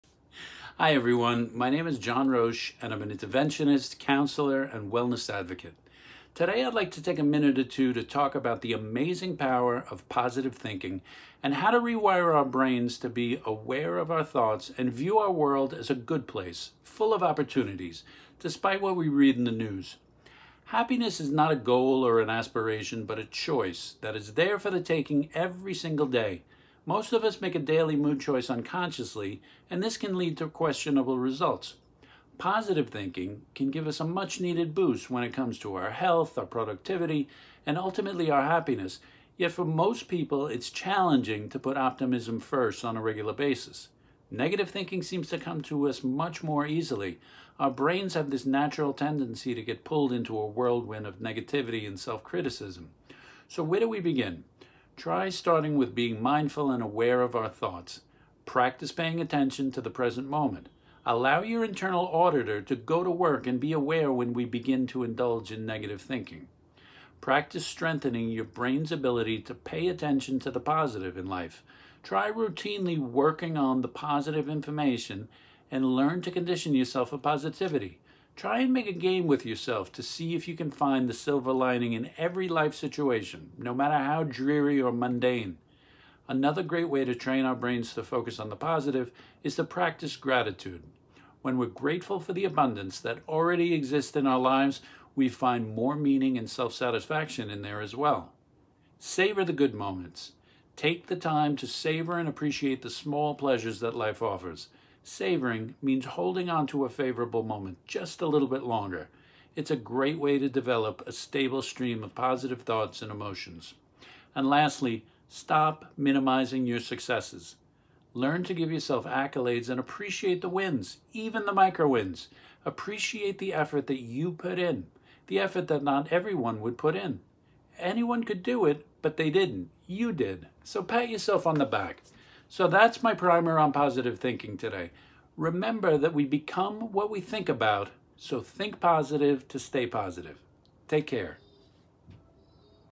The Power of Positive Thinking - John Roesch with some thoughts on positivity
A brief discussion on how thinking positive and avoiding negative thinking patterns and create change in your life.